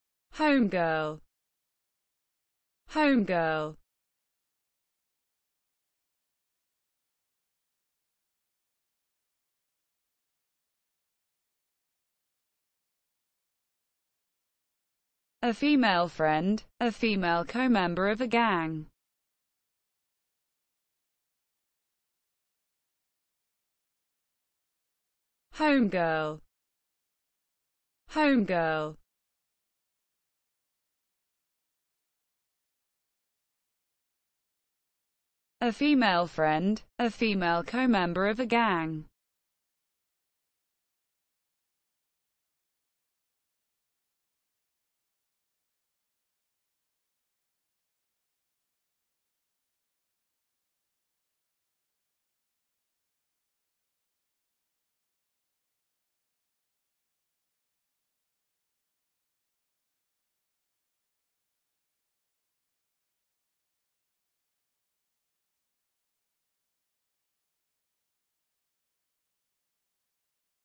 What does Homegirl mean?
What does Homegirl mean in English? Learn the meaning of the word Homegirl! How to Pronounce Homegirl. Expand your vocabulary, learn words.